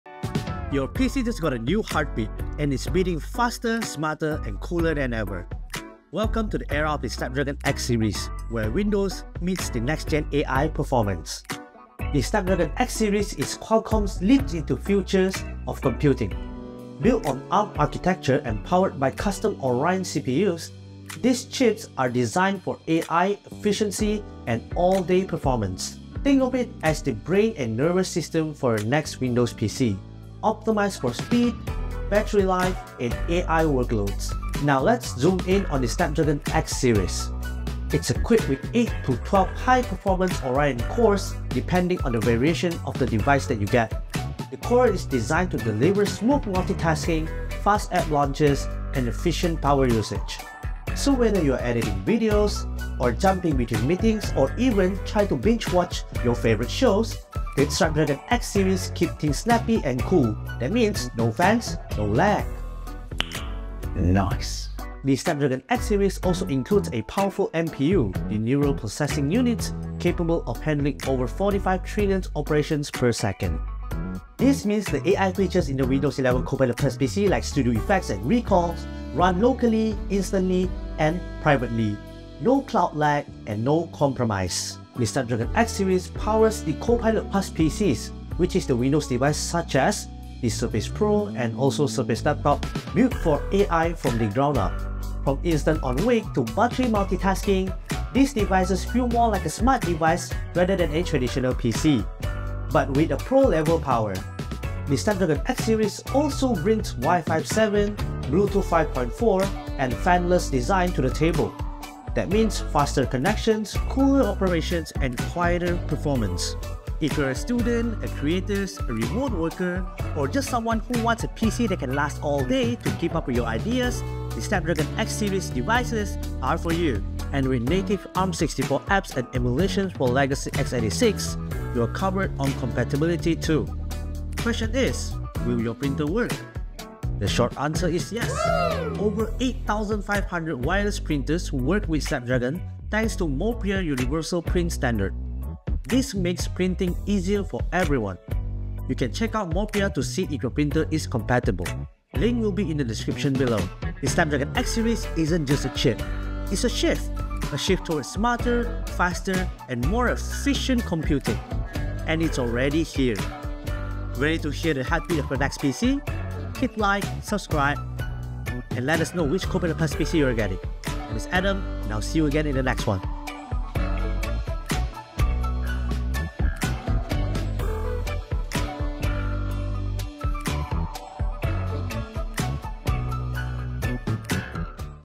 The New Era of Snapdragon X Series! | ExpertZone Malaysia
Welcome to the new era of Snapdragon X Series — where Windows PCs meet next-gen AI performance.
Discover how Qualcomm’s most advanced chip brings lightning-fast speed, incredible efficiency, and intelligent AI processing to Copilot+ PCs.